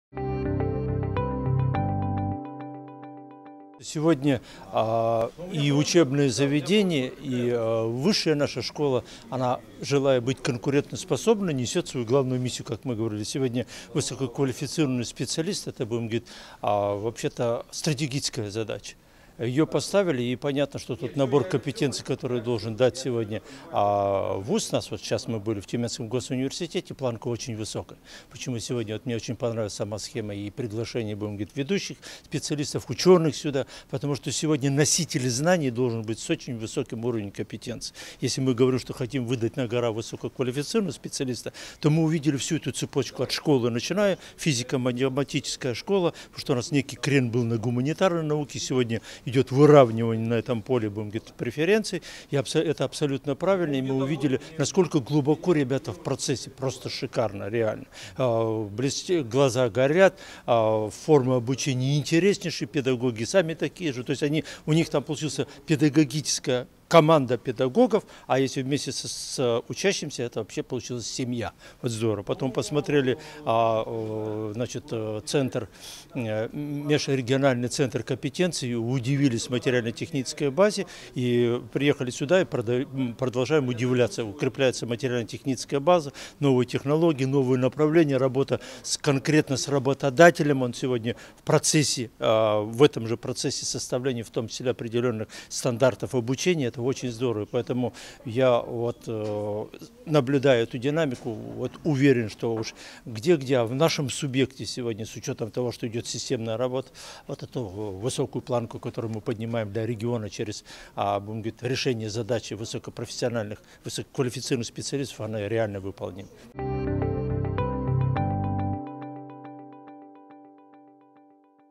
Виктор Рейн комментирует итоги выездного заседания комитета по социальной политике ТОД
Виктор Рейн, заместитель председателя Тюменской областной Думы, комментирует итоги выездного заседания комитета по социальной политике на тему «Подготовка высококвалифицированных кадров для нужд региона».